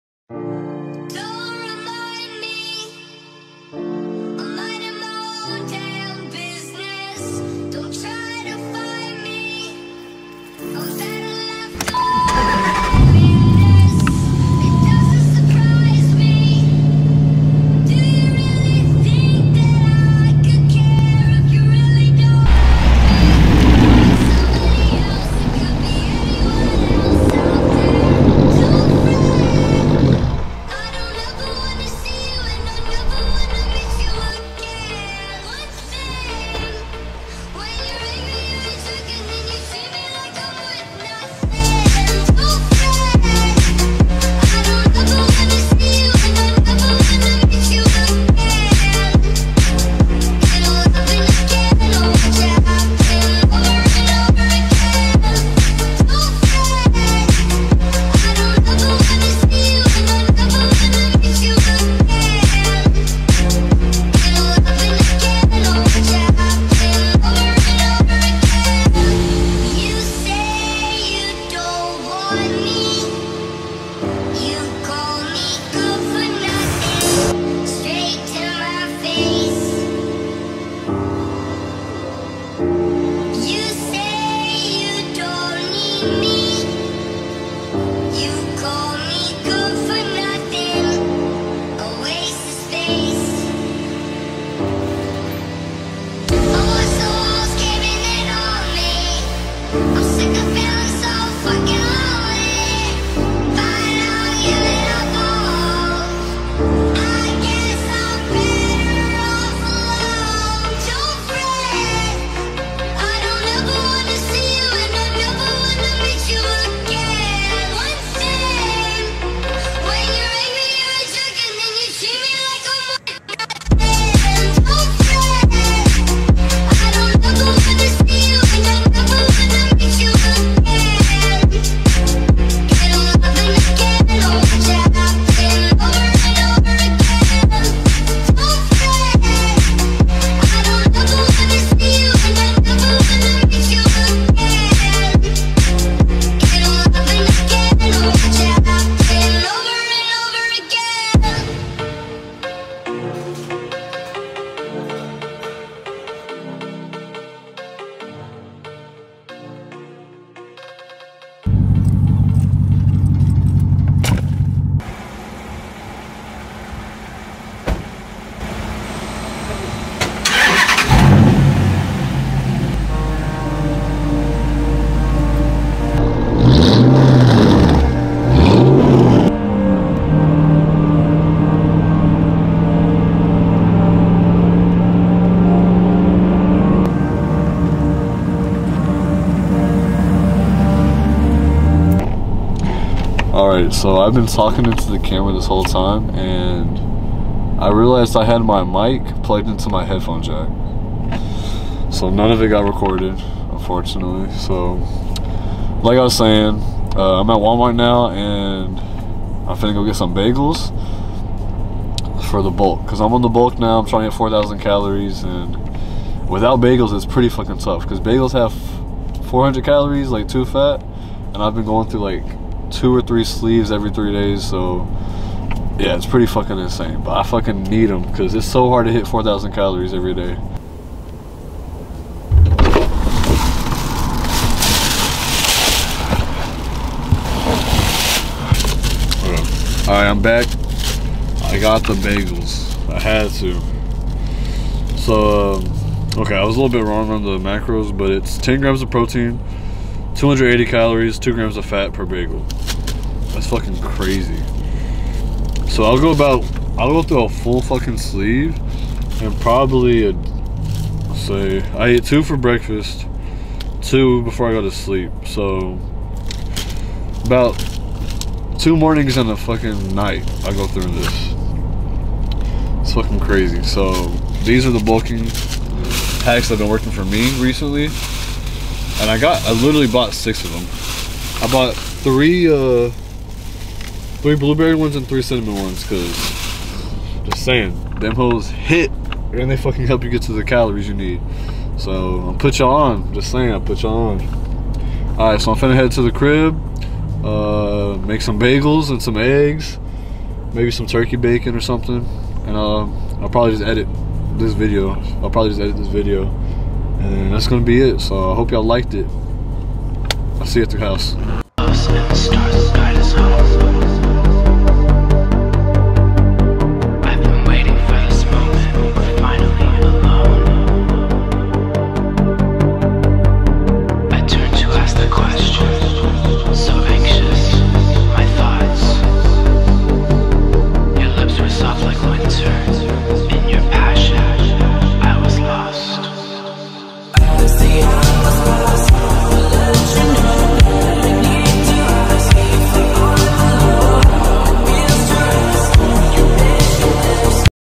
Little Gym Vlog Nun Special
Short video of us hitting legs didn't record much of it. Please like and sub🖤.